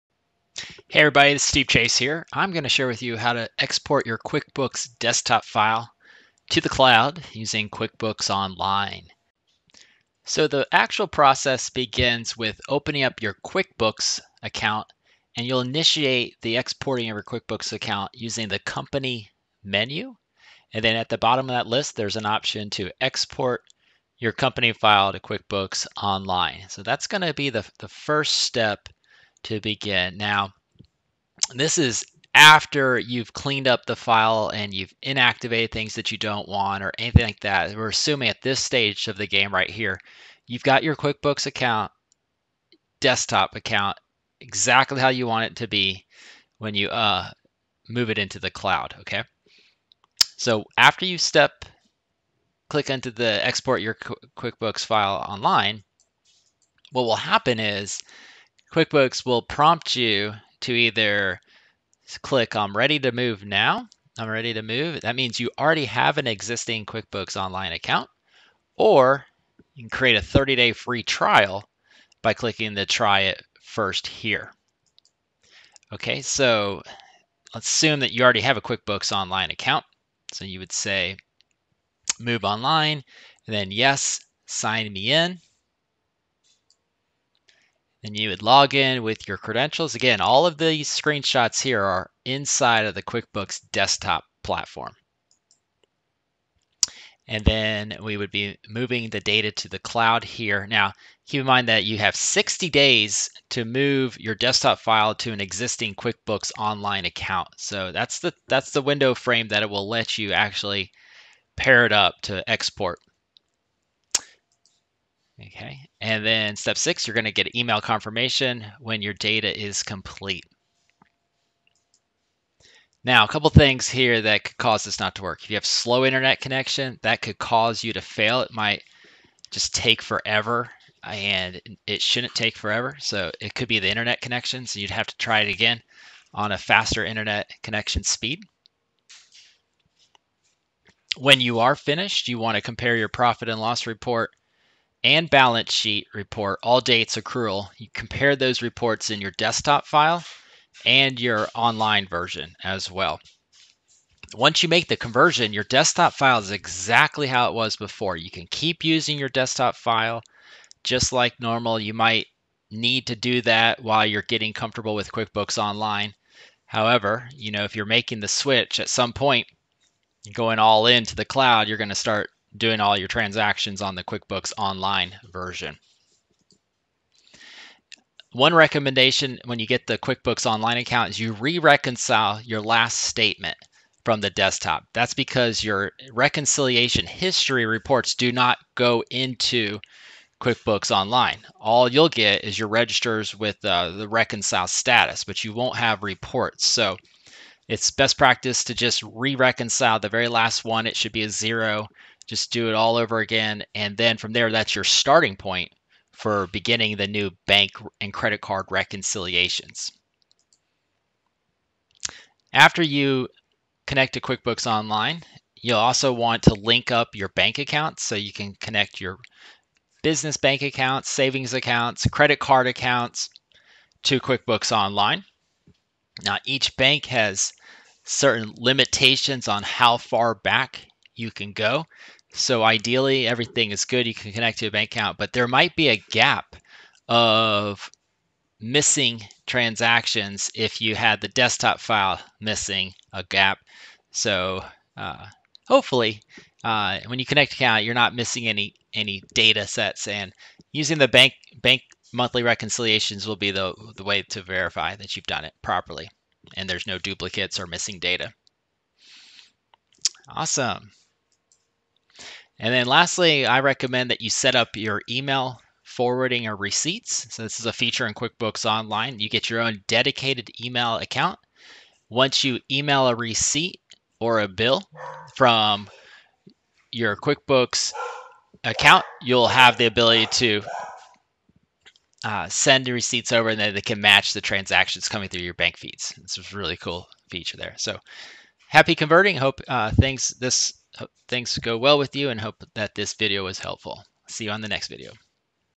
How to Export QuickBooks Desktop to QuickBooks Online
This video tutorial shows how to export QuickBooks desktop to QuickBooks Online.

Get peace of mind with backups to QBO with Rewind app